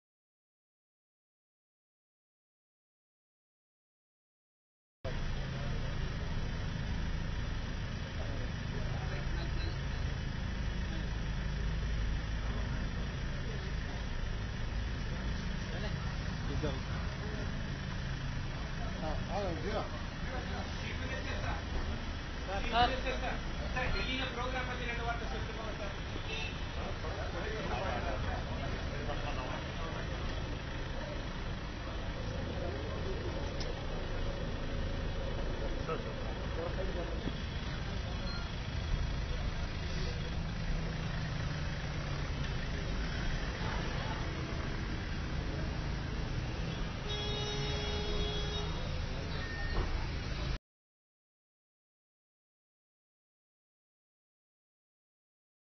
CM E Palanisamy arrives in New Delhi for NITI Aayog meeting - ANI
New Delhi, Apr 22 (ANI): Tamil Nadu Chief Minister E Palanisamy on Saturday arrived in the national capital.The Tamil Nadu CM has arrived here for the NITI Aayog meeting which is going to be held on 23rd April. There is a possibility that CM Palanisamy will meet the agitating farmers who are protesting at the Jantar Mantar since last month. Prime Minister Narendra Modi will chair the third meeting of NITI Aayog's Governing Council which will focus on the 15-year vision document to accelerate the country's economic development.